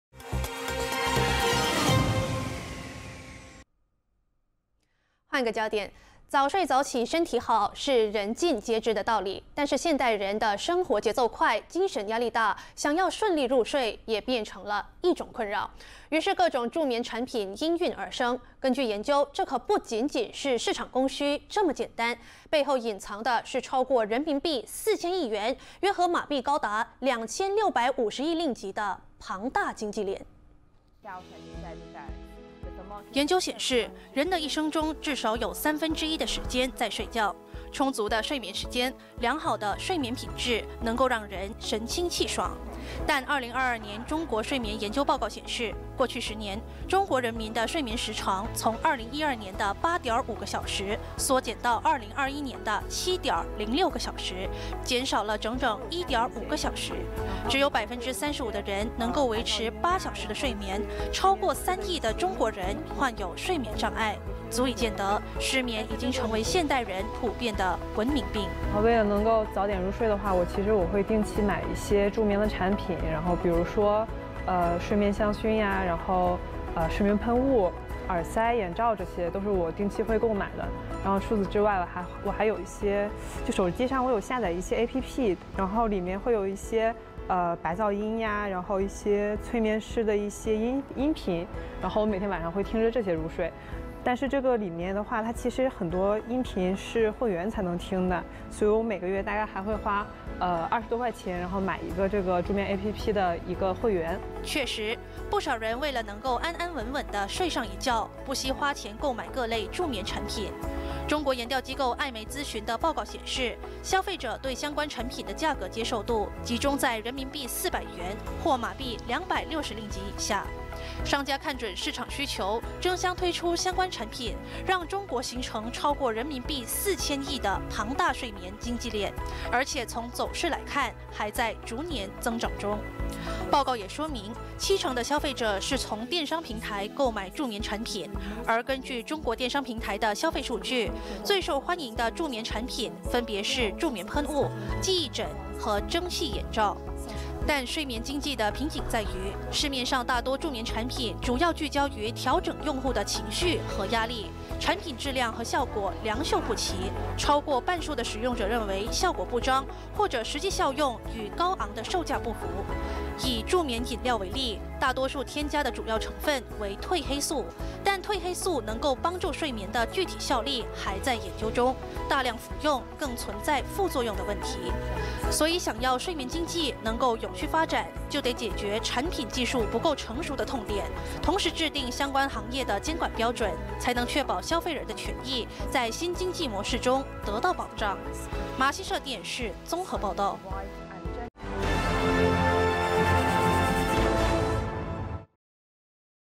20220326 中国3亿人患睡眠障碍 助眠产品热销成庞大经济链
早睡早起身体好是人尽皆知的道理，但是现代人的生活节奏快，精神压力大，想要顺利入睡也变成了一种困扰。

于是各种助眠产品应运而生，根据研究，这可不仅仅是市场供需这么简单，背后隐藏的是超过人民币4千亿元，约合马币高达2千650亿令吉的庞大经济链。